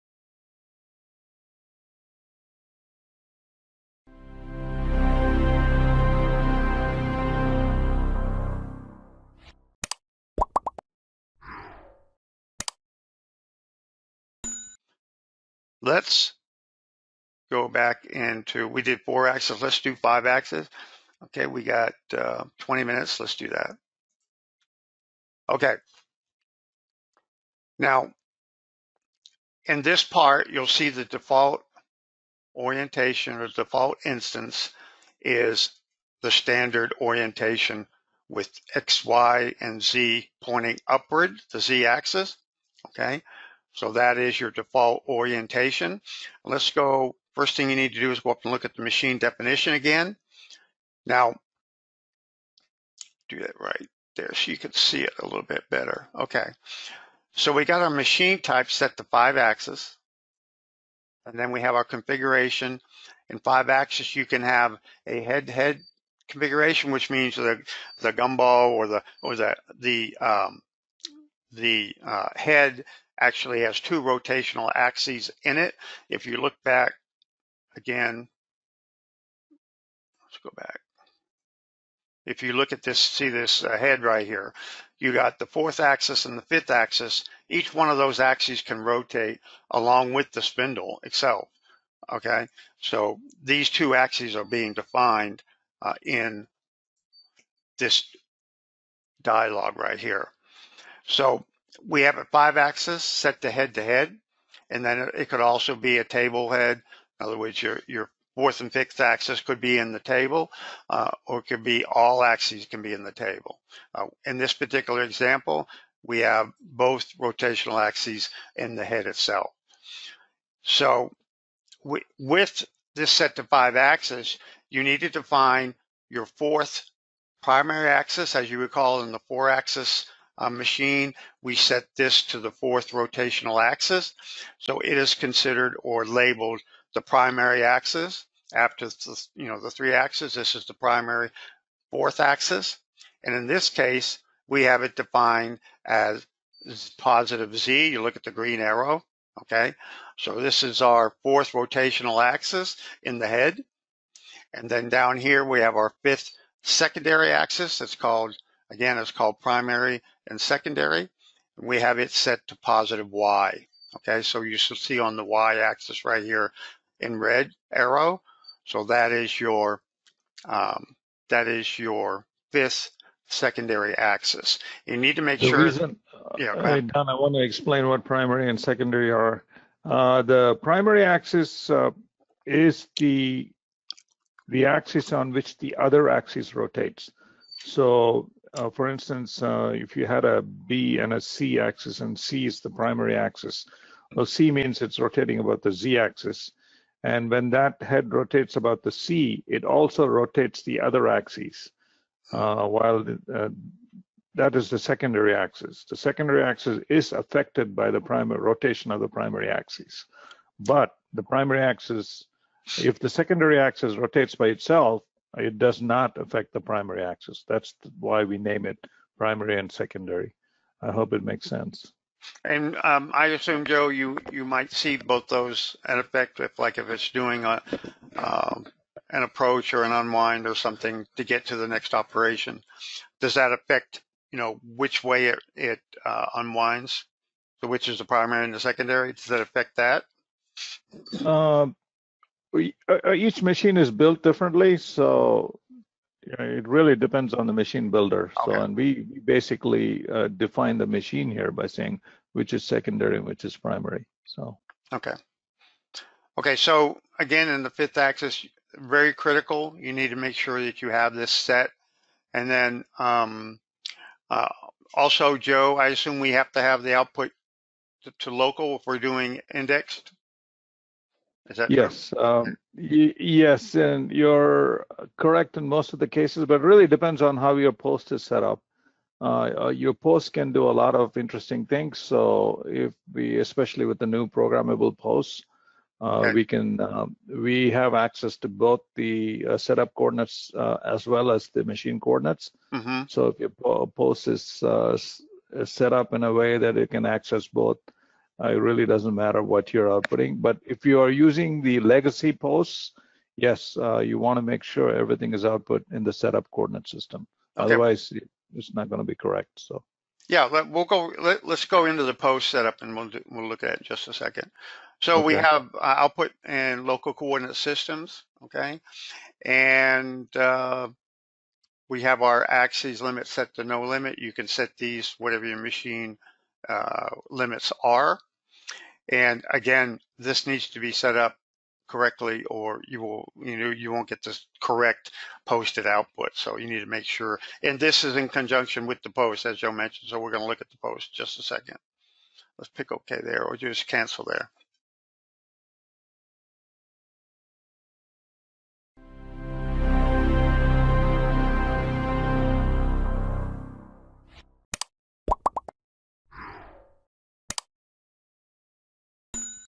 CAMJam #427: Indexed 5 Axis Machine Definition
Learn about post-processing Indexed 5 Axis Machine definitions in RhinoCAm and VisualCAM.